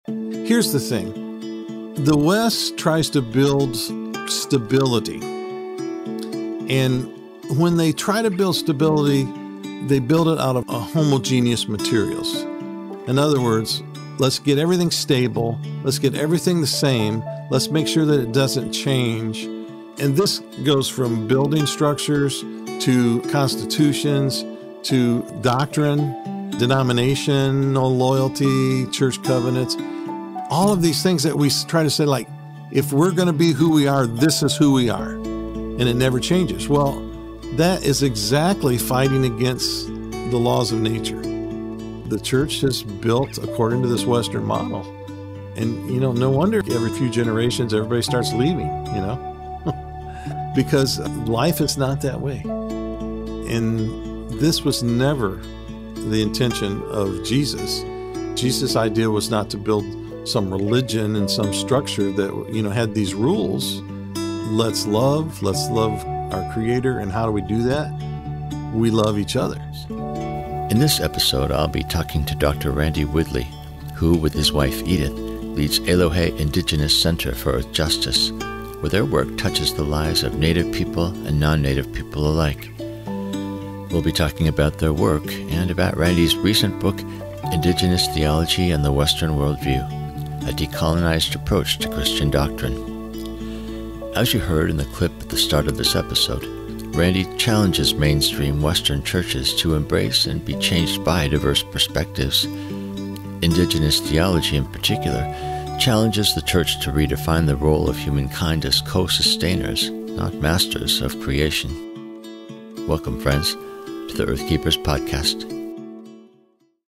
UNITY IN DIVERSITY: "Indigenous Theology and the Western Worldview" by Randy Woodley
In this episode, Forrest sits down with Dr. Randy Woodley who, with his wife Edith, leads Eloheh Indigenous Center for Earth Justice, where their work touches the lives of Native people and nonNative people alike. We’ll be talking about their work, and about Randy’s recent book, "Indigenous Theology and the Western World View: A Decolonized Approach to Christian Doctrine."  As you heard in the clip at the start of this episode, Randy challenges mainstream Western churches to embrace and be changed by diverse perspectives; indigenous theology in particular challenges the church to redefine the role of humankind as co-sustainers—not masters—of creation.